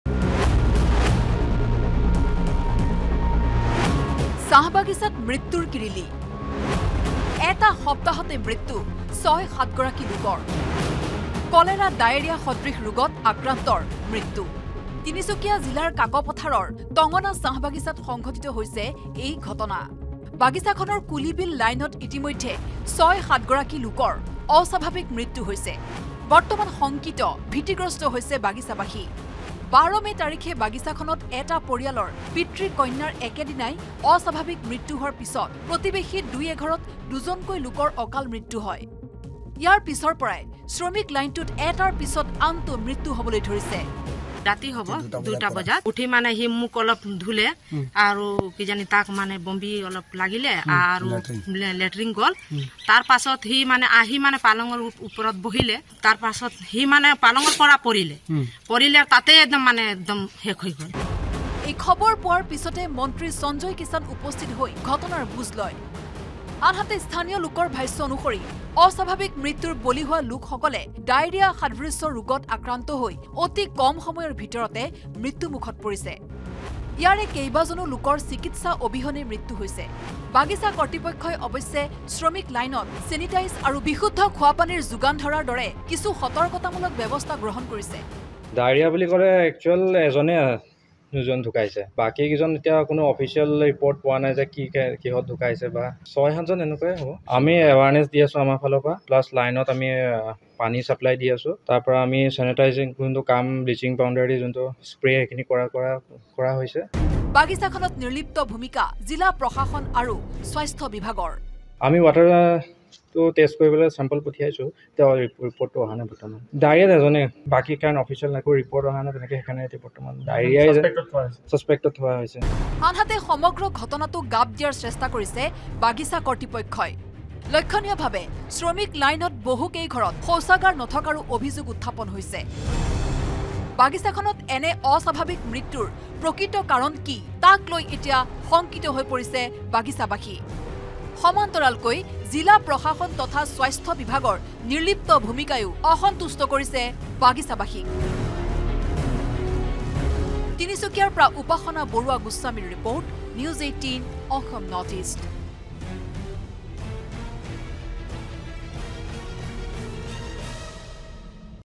Tinsukia News | কলেৰা-ডাইৰীয়া সদৃশ ৰোগত এটা সপ্তাহত মৃত্যু কেইবাগৰাকী লোকৰ N18V
কাকপথাৰৰ টঙনা চাহ বাগিচাত কলেৰা আৰু ডায়েৰীয়া সদৃশ ৰোগৰ প্ৰাদুৰ্ভাৱ। চাহ বাগিচাখনত ১১গৰাকী লোকৰ কলেৰা আৰু ডাইৰীয়াসদৃশ ৰোগত মৃত্যু। এসপ্তাহতে বাগিচাখনৰ কুলীবিল লাইনত ৭ গৰাকী লোকৰ মৃত্যু হৈছে এই ৰোগত